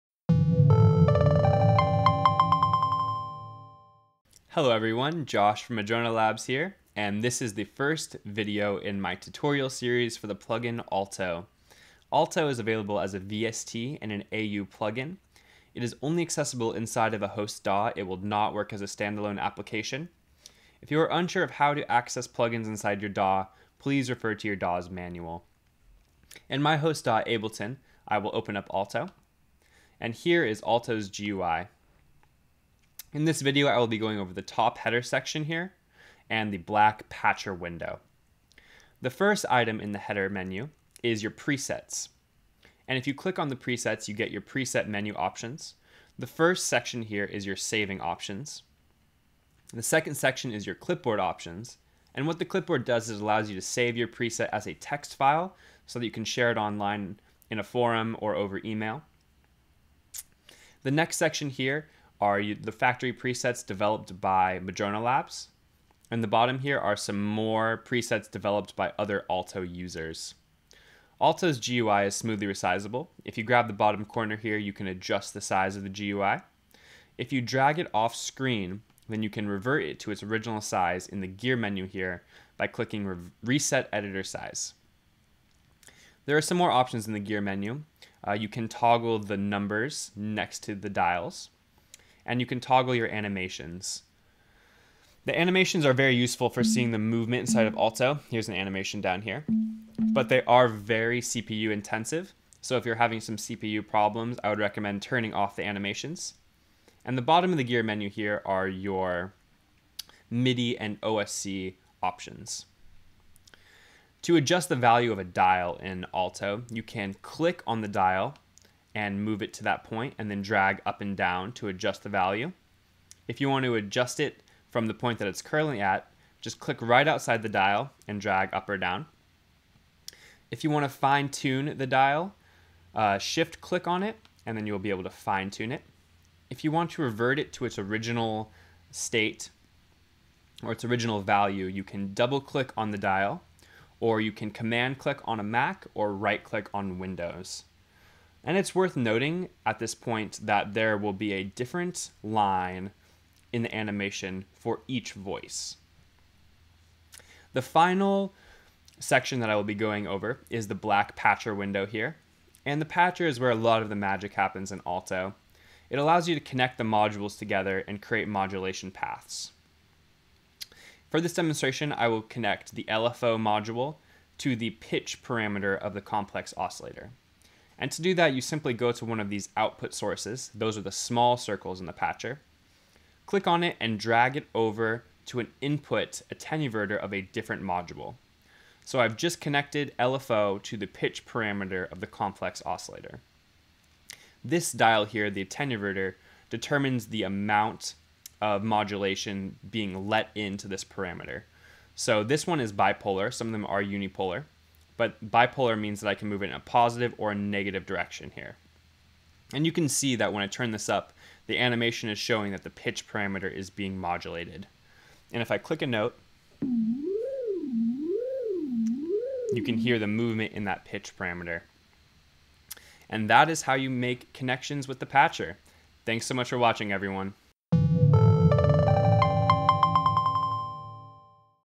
Aalto Tutorial #1: Header and Patcher
One in a series of tutorials on the Aalto software synthesizer by Madrona Labs.